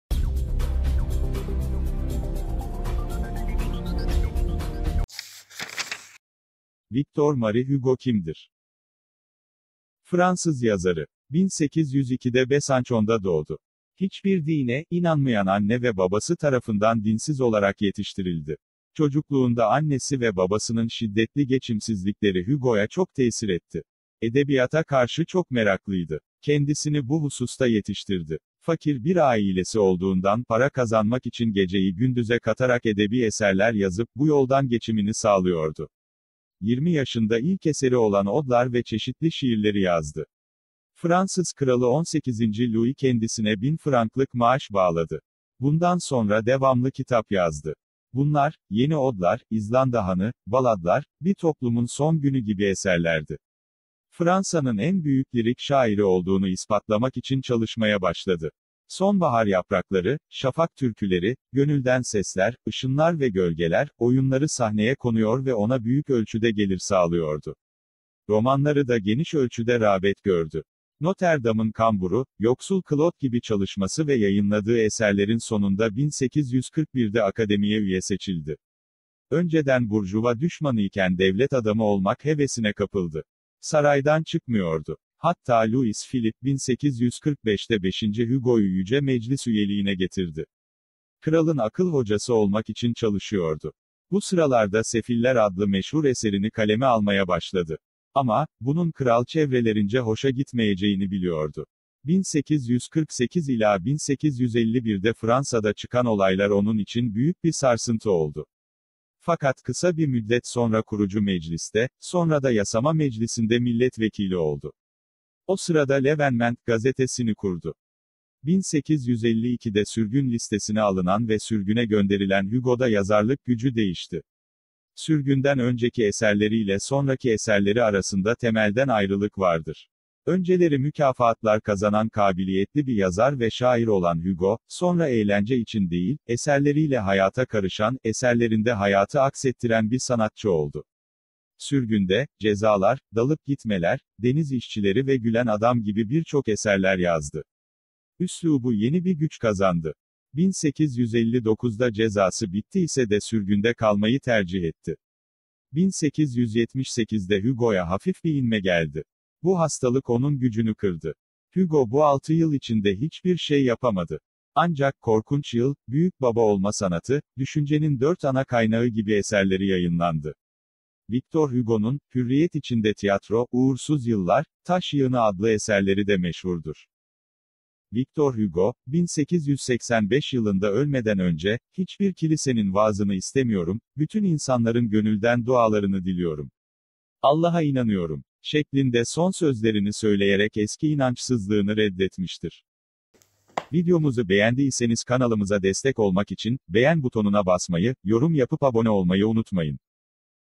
Hugo Kimdir? (Victor Marie Hugo)
Videomuz, Victor Marie Hugo  hakkında ansiklopedik genel bilgi içermektedir. Ayrıca, merak ettiğiniz konular hakkında yorum bırakırsanız, kaynaklarımız ölçüsünce, video hazırlamak için elimizden geleni yaparız.

Kaynak: Rehber Ansiklopedisi